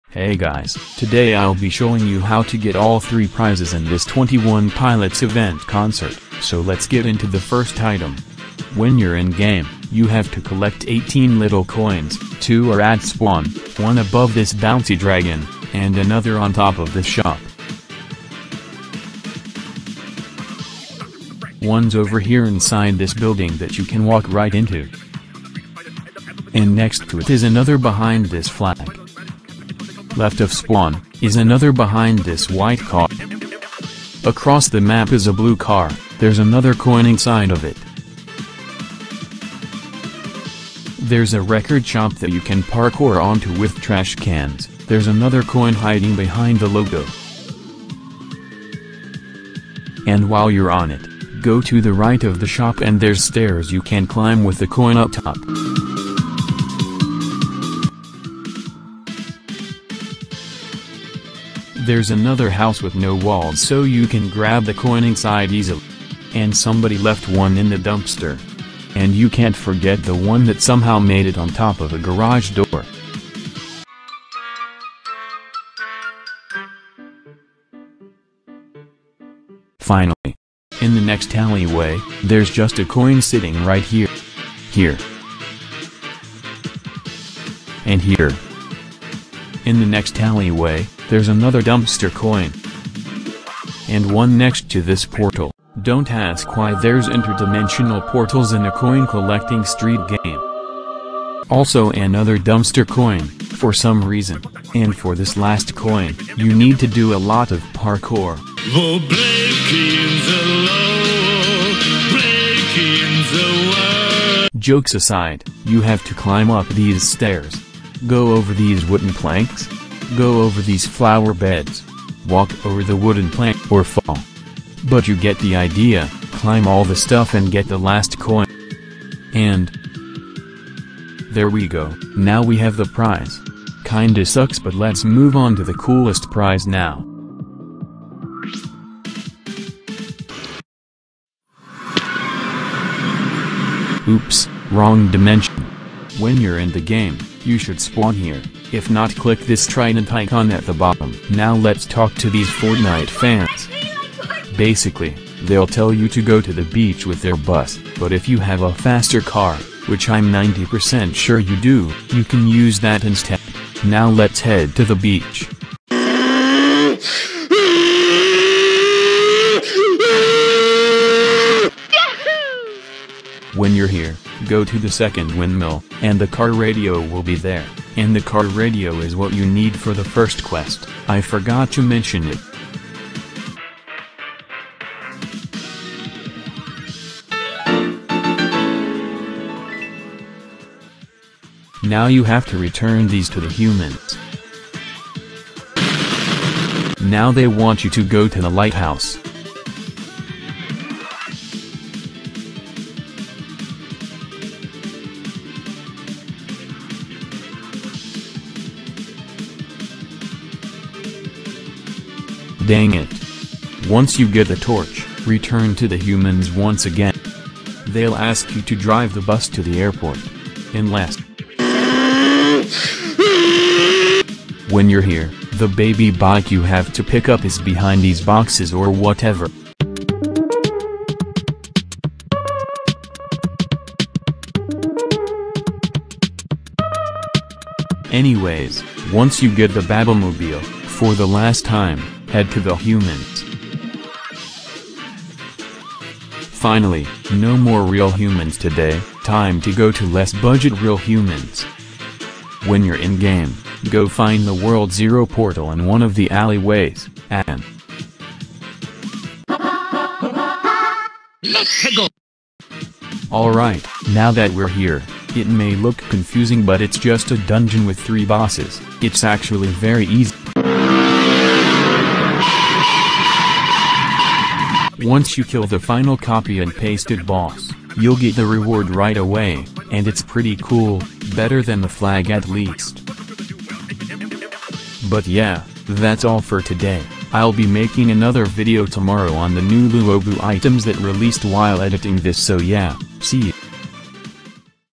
How to get ALL FREE ITEMS in the Twenty One Pilots Event!!!
3 items, 2 good looking ones to choose from, which will you pick?